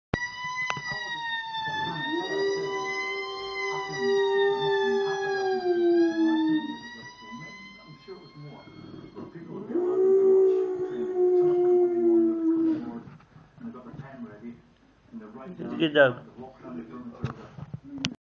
layla vs the police siren
Layla seems to think police sirens are dogs howling.  She was on heat at this point so was acting very strange, moaning all the time, even in her sleep that sort of thing.  Anyway she heard a police siren on the TV and started howling, we had never seen her do it, which lead to us using our favourite video site to encourage this behaviour...